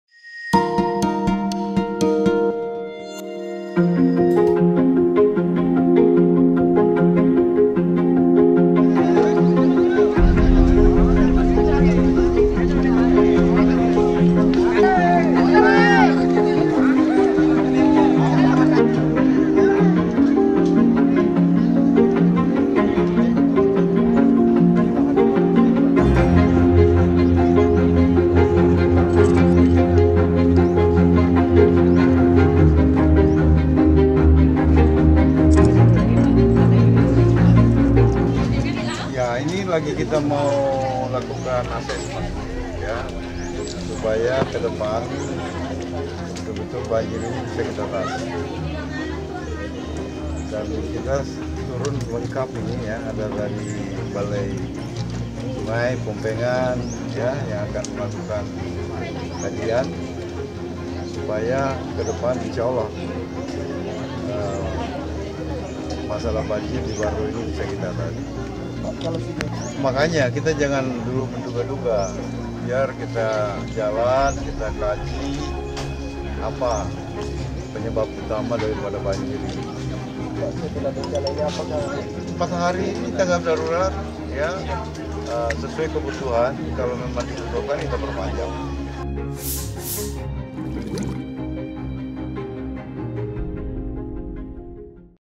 Gubernur Tinjau Lokasi Banjir di Barru
SULSELSATU.com, BARRU – Gubernur Sulsel, Nurdin Abdullah meninjau lokasi banjir di Kabupaten Barru, Sabtu (29/12/2018). Dalam kesempatan itu, Bupati Barru, Suardi Saleh memaparkan, kerusakan dan kebutuhan bagi seluruh korban banjir yang terjadi sejak hari Jumat (28/12/2018) kemarin.

Videographer: Asriadi Rijal
Video Editor: Andi Hermanto
Editor: Awang Darmawan